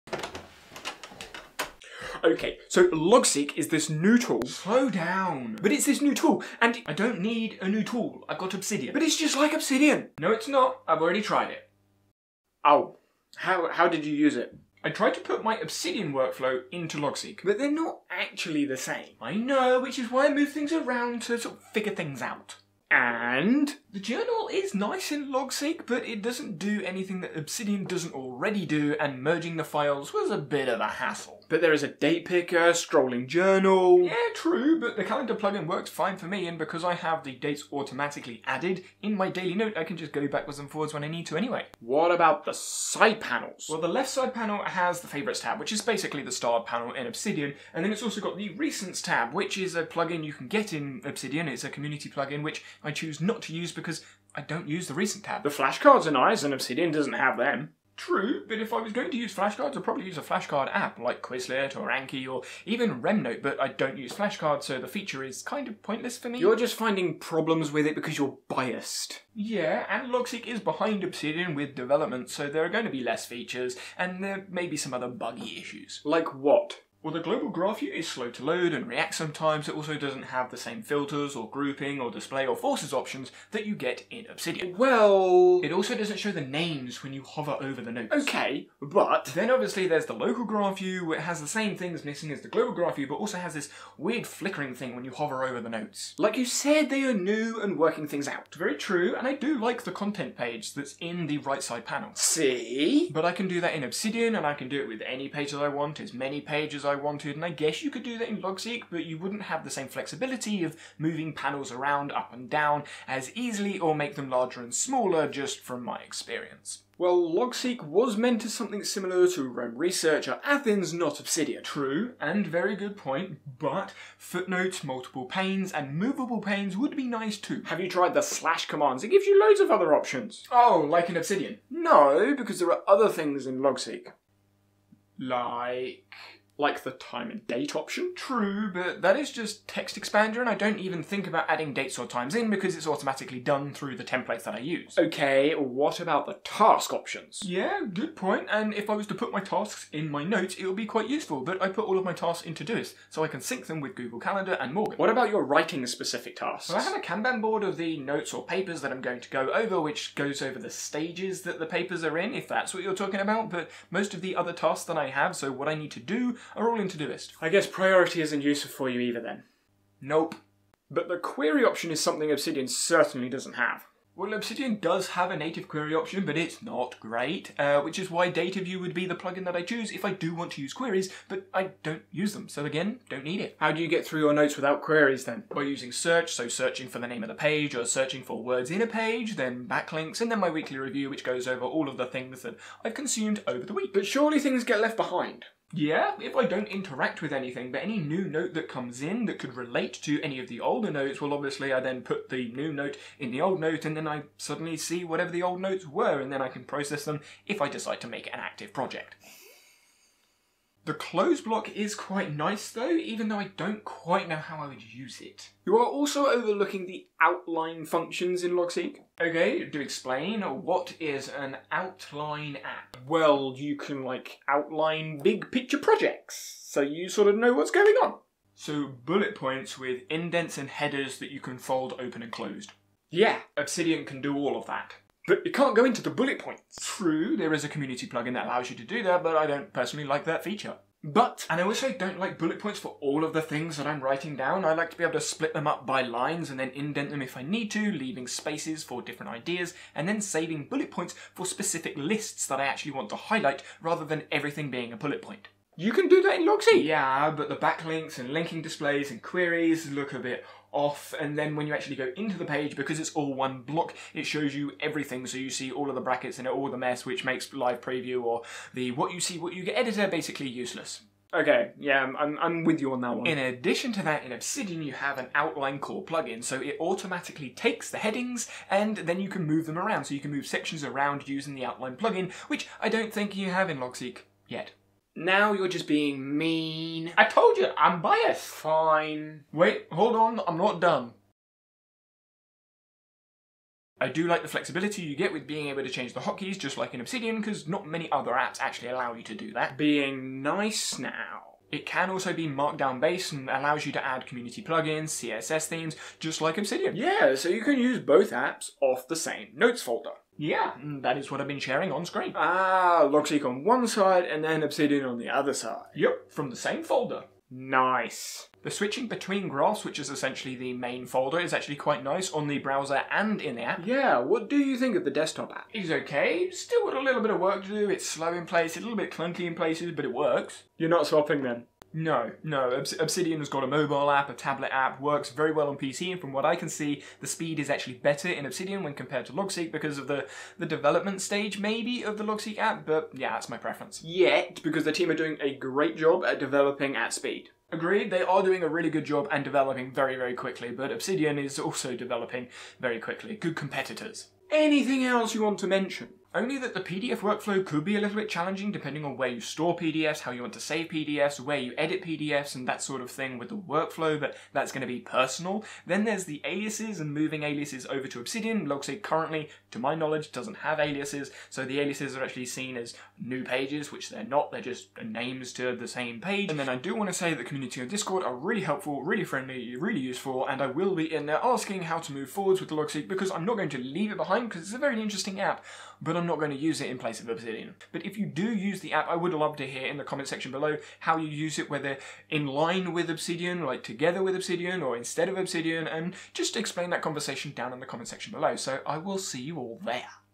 Comparing Logseq and Obsidian
I tried using Logseq browser and an app version for a week instead of my normal Obsidian vault and this is what I found.